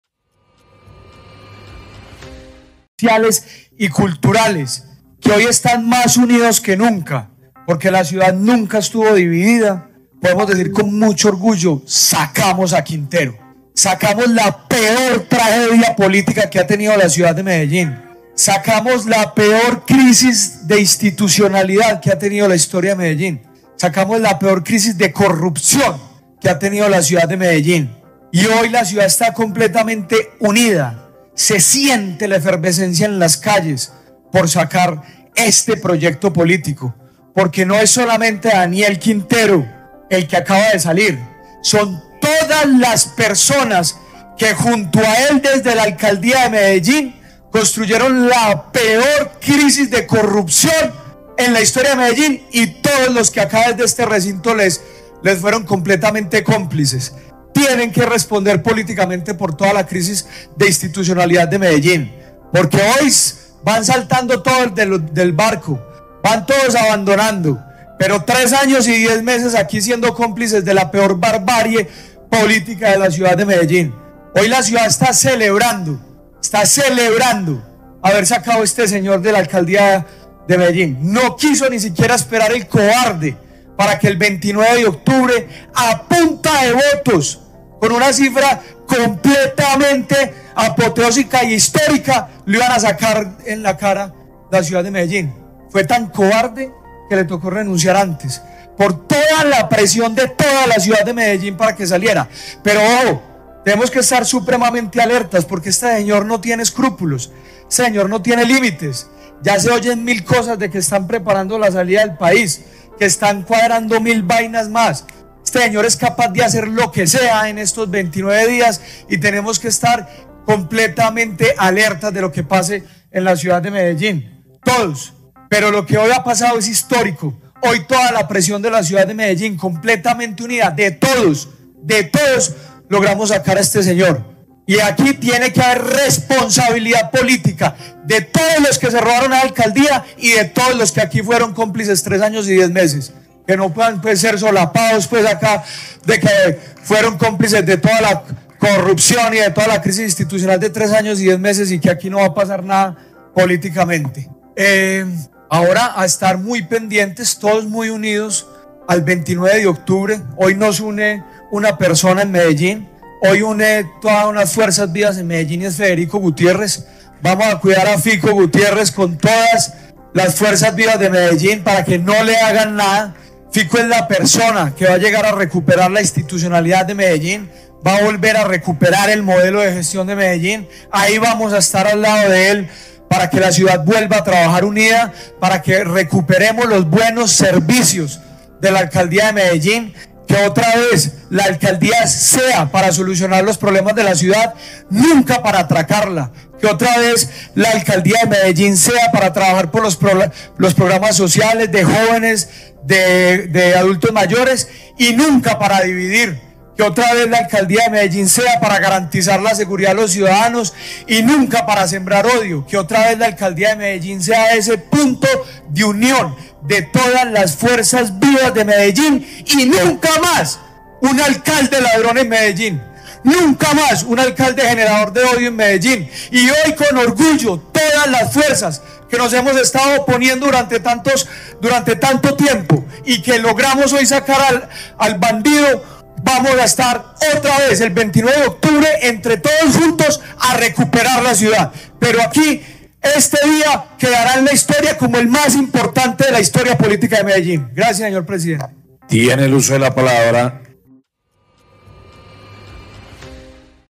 📢 DANIEL QUINTERO: LA PEOR TRAGEDIA INSTITUCIONAL EN LA HISTORIA DE MEDELLÍN
Sacamos la peor tragedia institucional de la historia de Medellín; ¡sacamos a Quintero!
Quedarás Quintero Calle en la historia como el peor alcalde de Medellín.
SEBASTIAN LÓPEZ